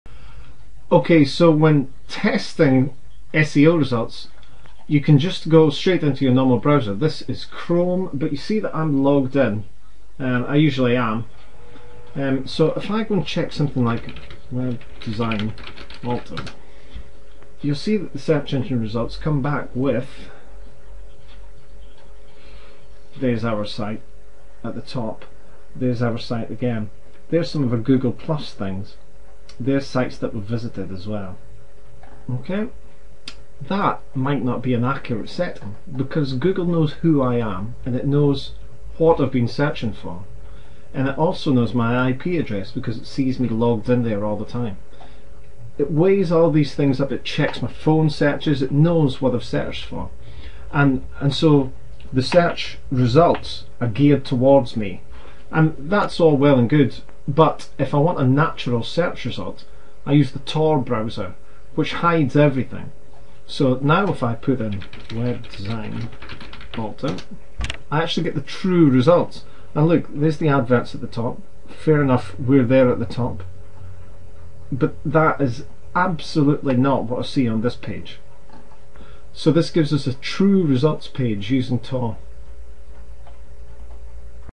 True SEO results using TOR Browser
Google make our lives easy by helping with our individual searches.  They take into account our previous searches, location, sites visited, etc...   But what if you want to find out the true search results?  For that we use the TOR browser which hides all that information.